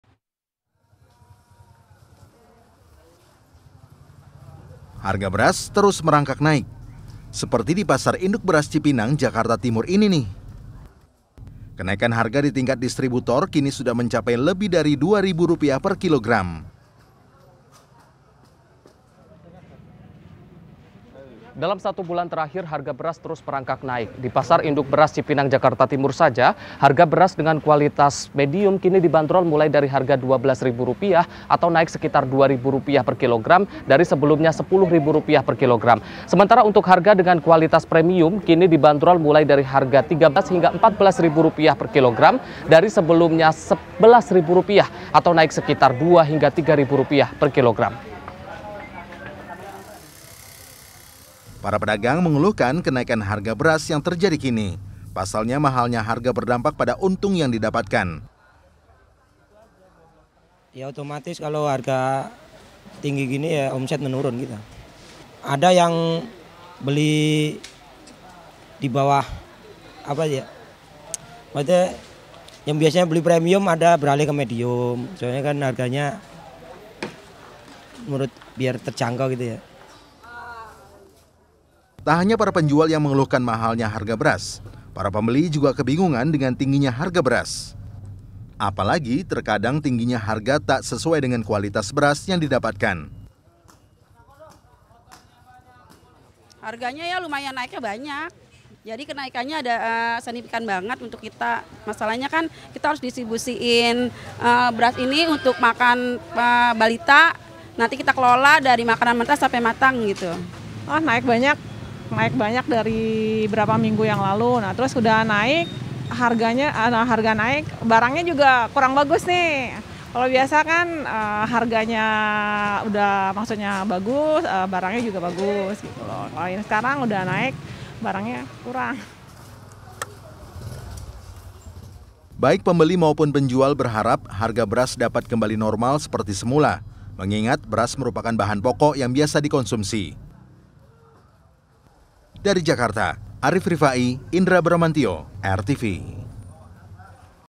Harga Beras di Pasar Induk Cipinang Melonjak
Dampak dari musim kemarau dan kekeringan. harga beras di Pasar Induk Cipinang, Jakarta Timur melonjak. Kenaikan terjadi di semua jenis.